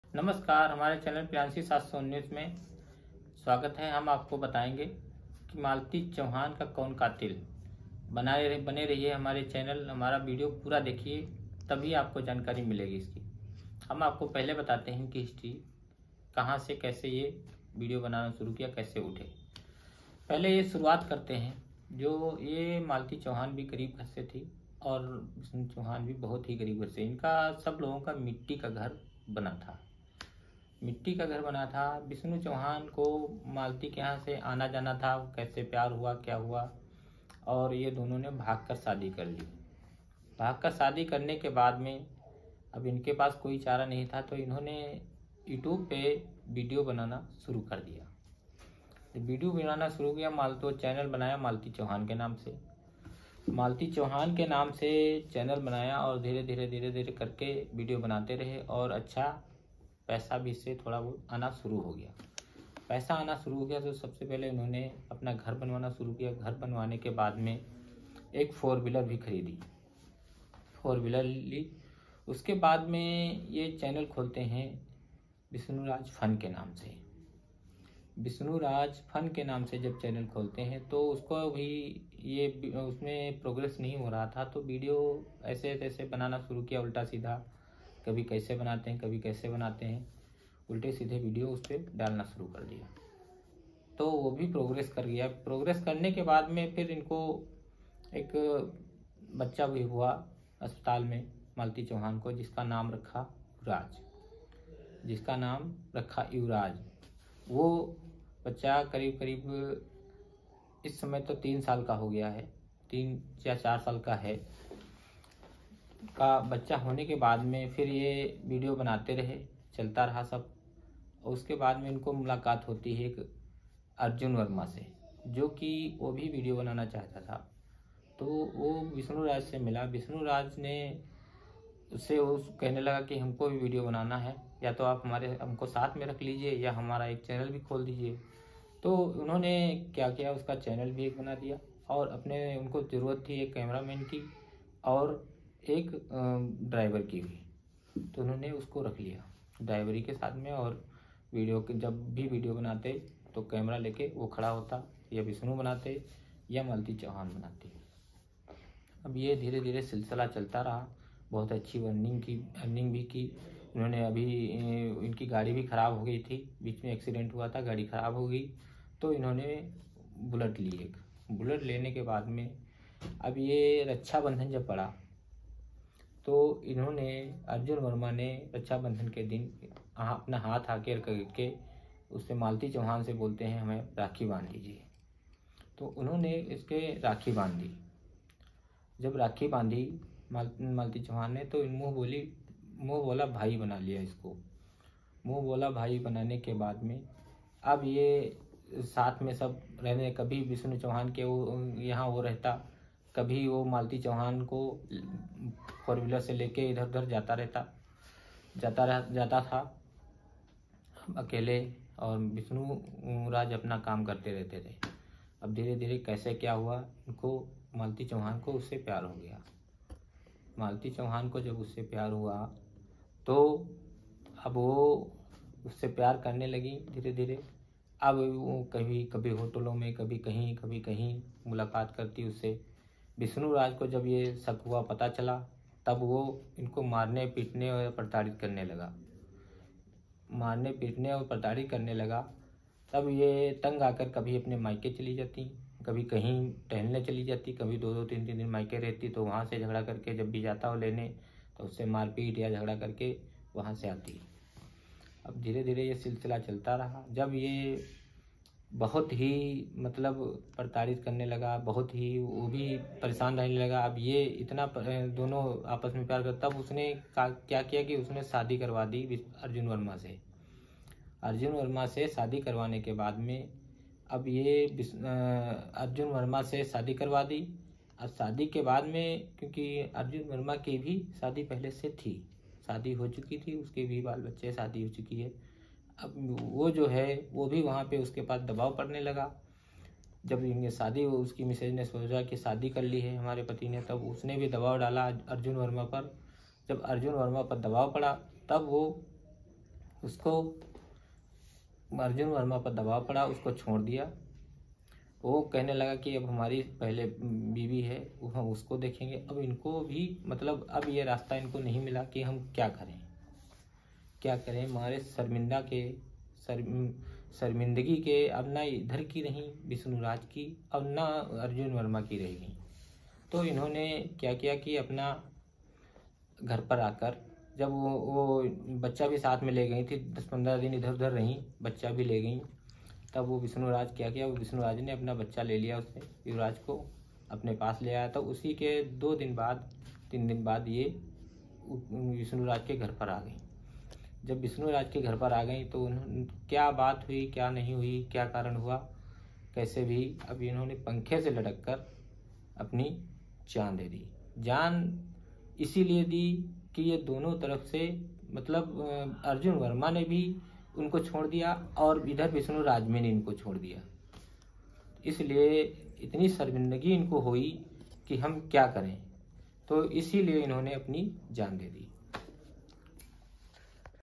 😱 MALTI CHAUHAN OR VISHNURAJ KI STORY KYA HAI || PR719
मालती चौहान को किसने मा* *रा जानिए पुरा सच! Vishnuraj Maltichauhan Ki Story | PR719

What happened to Bhojpuri's famous YouTuber Malti Chauhan. An attempt has been made to tell in this story. @MaltiChauhanFunofficial @maltichahuan ma#maltichauhan